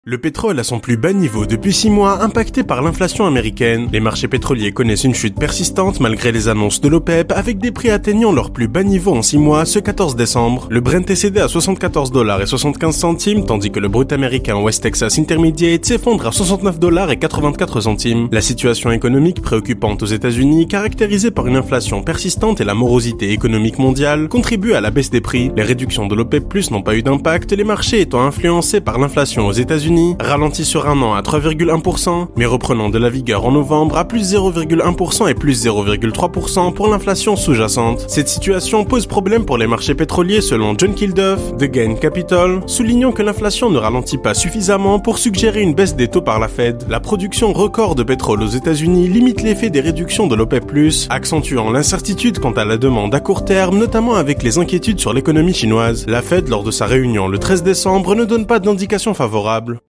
Le pétrole à son plus bas niveau depuis 6 mois, impacté par l'inflation américaine
Les marchés pétroliers sont entrés dans une spirale de chute successive. Les prix du pétrole ne cessent de dégringoler malgré les annonces de l'Organisation des pays producteurs du pétrole (OPEP). La tendance baissière n'est donc pas conjoncturelle et s'installe dans le temps. En effet, un nouveau facteur est venu compliquer encore plus la situation.